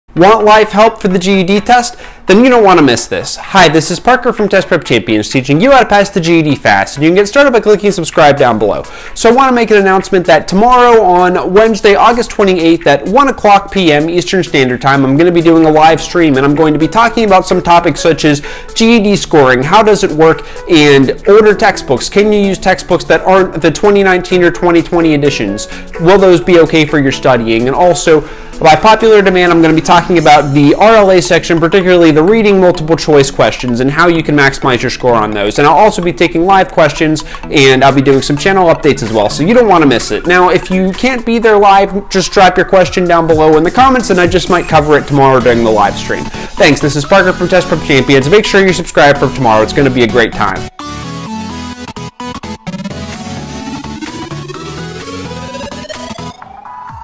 Announcing: Live GED Help (8/28/19 @ 1pm EST)
Join me live tomorrow here on YouTube at 1pm EST for my weekly live stream! If you can't be there live, drop your question down below in the comments, and I just might answer it on the live stream!

Some of the topics I'll be talking about are:
-GED scoring
-New vs old textbooks
-RLA multiple-choice and reading tips